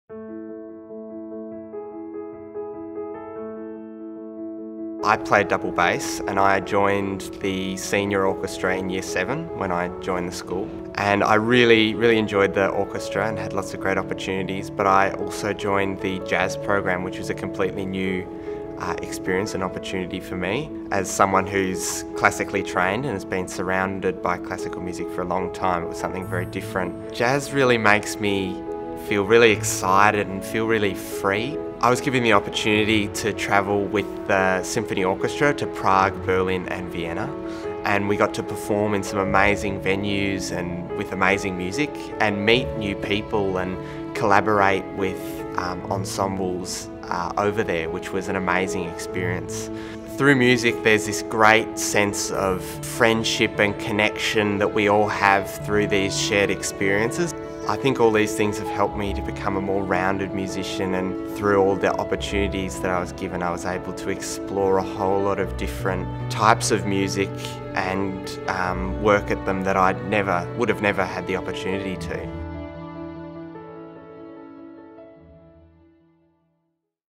Luca Arcaro
An accomplished double bass player, 2020 Year 12 student, Luca Arcaro talks about his experiences beyond the classroom at Melbourne Grammar School.

Luca was Captain of Music at Melbourne Grammar in 2020 and was a key member of the Symphony Orchestra, Jazz Ensemble and Big Band, among other groups, in his final year at the School. Luca was also a member of the Australian Youth Orchestra in 2020 and been invited to participate again in 2021. In addition, Luca was a member of the Melbourne String Ensemble from 2015 to 2019.

This video was recorded in December 2020 at the conclusion of the VCE exam period prior to the release of results.